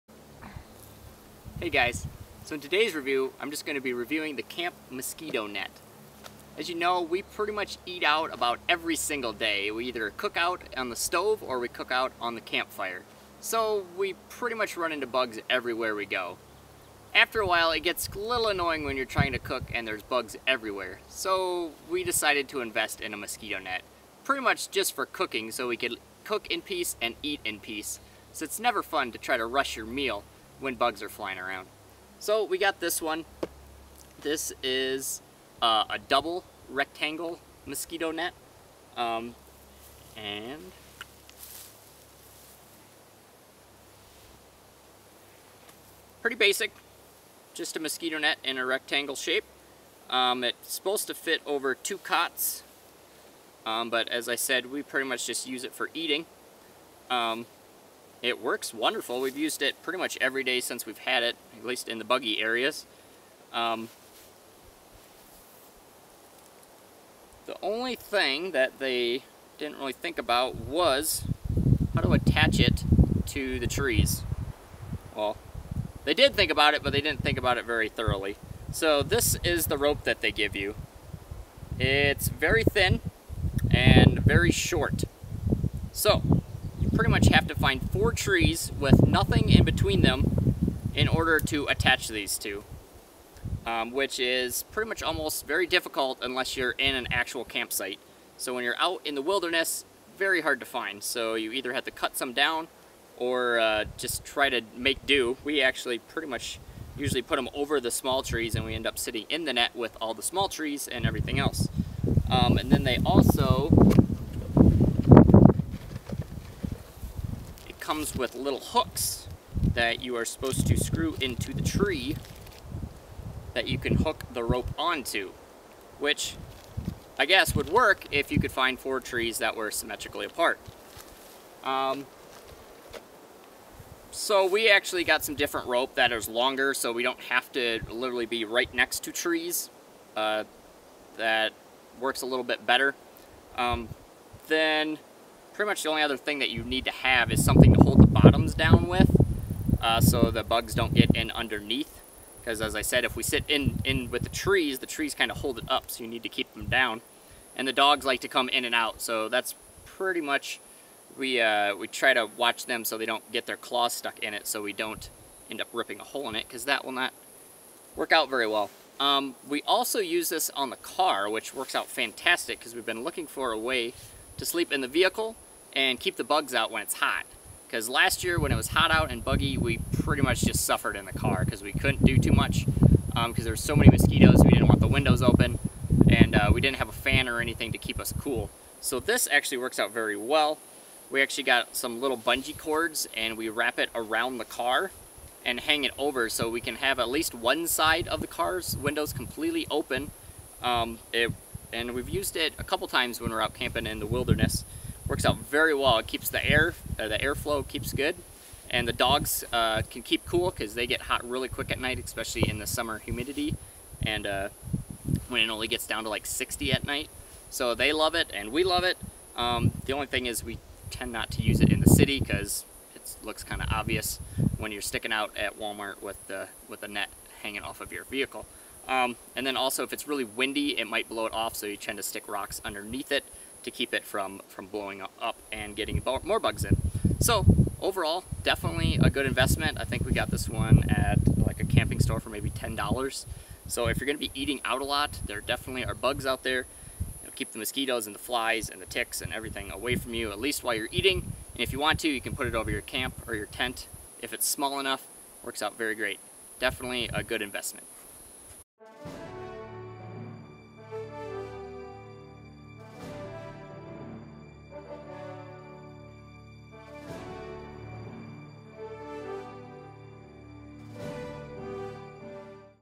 Do Bug Nets Really Work?
If you plan to live on the road & move with the seasons or maybe you'll just be doing some longterm travel in places that have a higher bug population than people, we have a must have piece of gear here - a bug net! Wondering if these things are worth the money? Do they actually keep the bugs out? How often will you use it? Want ideas on other ways to use it besides just an outdoor bug tent? We've got it covered in this gear review. Enjoy & hope it helps you keep the red spots down.